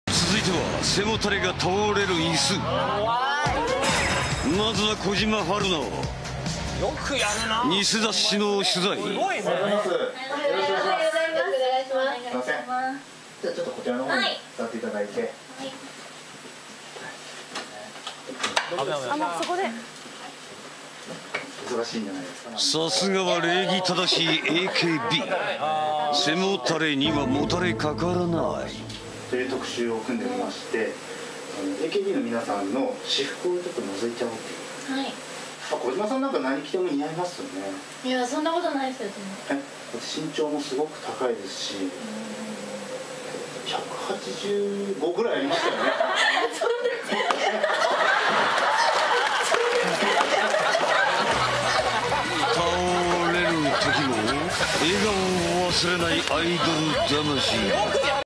Kojima Haruna vs. Chair
Kojiharu is the victim of a prank chair